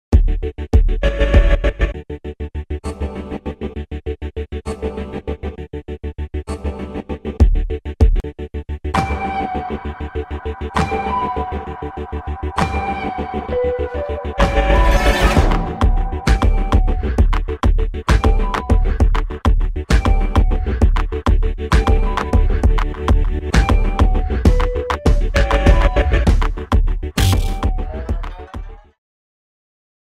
The new A-Class
It is designed for you. From the athletic contours to traditional styling of the A-Class – all that's needed is your personal touch. See it live at The new A-Class Launch, Star Arena - Senayan City, 5-7 October 2018.